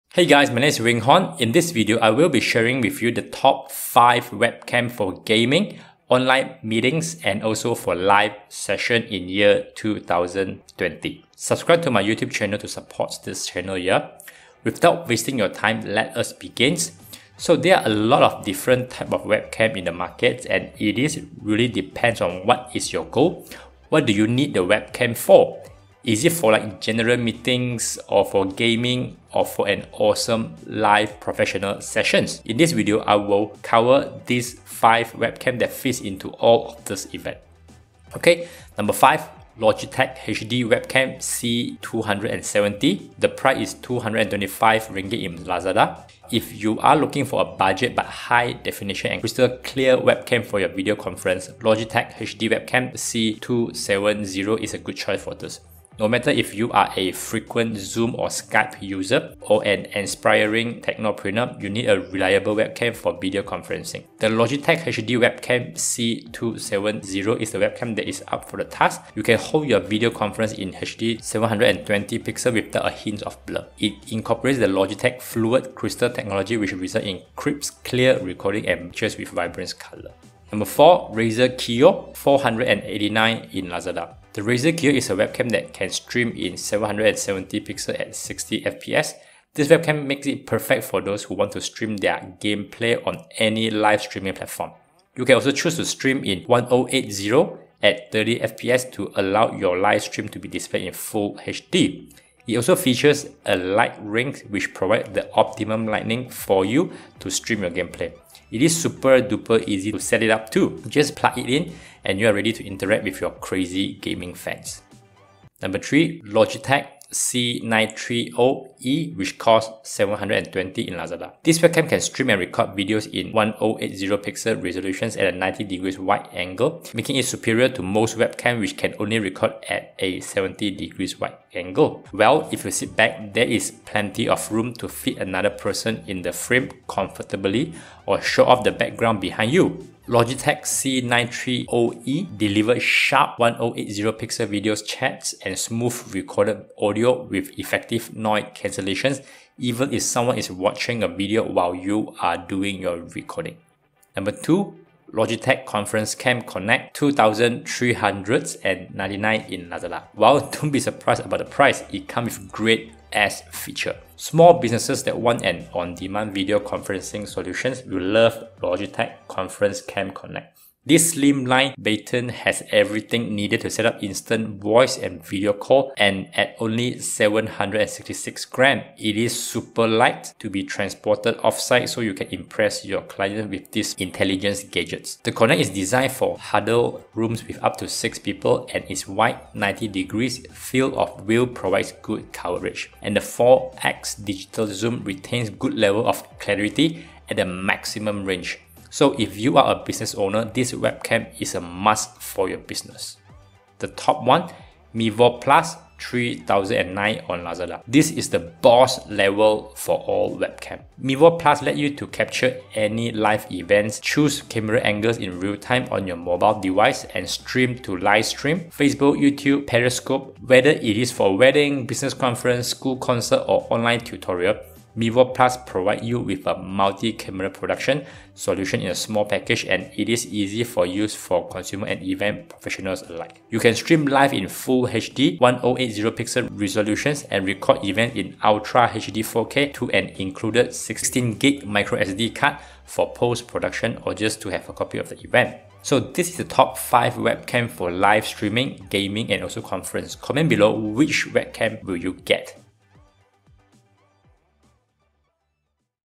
TOP 5 LIVE STREAMING WEB CAM FOR GAMING , LIVE STREAMING , MEETING 2020
#5 Logitech HD Webcam C270 RM 225
Hi guys my name is Weng Honn , in this video I will be sharing the top5 web cam for gaming , online meeting and also for live session 2020
Without wasting your time , lets us begin.
There are a lot of different type of web cam in the market , it is really depends on what is your goal , what do you need the web cam for , is it for general meeting use , for gaming or for an awesome live session .
In this video I will cover 5 web cam that fits into all of this event.
#5 Logitech HD Webcam C270 RM 225 in lazada
If you are looking for a budget but high defination and crystal clear web cam for your video conference , Logitech HD Webcam c270 is a good choice for this.
No matter if you’re a frequent Zoom or skype user or an aspiring technopreneur, you need a reliable webcam for video conferencing. The Logitech HD Webcam C270 is the webcam that’s up for the task – you can hold your video conference in HD720p without a hint of blur.
It incorporates the Logitech Fluid Crystal Technology which results in crisp-clear recording and breathtaking pictures with vibrant colours  .
#4 Razer Kiyo  RM 489 in Lazada
The Razer Kiyo is a webcam that can stream in 720p at 60fps. This webcam makes it perfect for those who want to stream their gameplay on any live streaming platform. You can also choose to stream in 1080p at 30fps to allow your live stream to be displayed in full HD.
It also features a Light Ring which provides the optimum lighting for you to stream your gameplay.  It’s super-easy to set up too - just plug it in, and you’re ready to interact with your crazy gaming fans!
#3 Logitech C930e   RM720.00
This webcam can stream and record videos in 1080p resolution at a 90º wide angle – making it superior to most webcams which can only record at a 70º wide angle. If you sit back, there's plenty of room to fit another person in the frame comfortably or show off the background behind you.  Logitech's c930e delivers sharp 1080p video chats and smooth recorded audio with effective noise cancellation, even if someone is watching a video while you are filming!

#2 Logitech ConferenceCam Connect RM2399 in lazada
Don’t be surprise about the price , it come with great feature.
Small businesses that want an on-demand video conferencing solution will love Logitech's ConferenceCam Connect. This slim-line baton has everything needed to set up instant voice and video calls and at only 766g, it's light enough to be transported off-site so you can impress your clients.
The Connect is designed for huddle rooms with up to six people and its wide 90-degree field of view provides good coverage. Its auto-focus worked fine during testing and the 4X digital zoom retains good levels of clarity at the maximum range.
If you are a business owner this web cam is a must for your business!

#1 Mevo Plus  RM3009 on Lazada
This is the boss level of all web cam.
Mevo Plus lets you to capture any live event, choose camera angles in real time on your mobile device, and stream to Livestream, Facebook, YouTube, Periscope, or Twitch.
Whether for a wedding, business conference, school concert, or online tutorial, Mevo Plus provides you with a multicamera production solution in a small package, and it's easy to use for consumers and event professionals alike.
You can stream live in Full HD 1080p resolution, and record events in Ultra HD 4K to an included 16GB microSD card for post-production or just to have a copy of the event.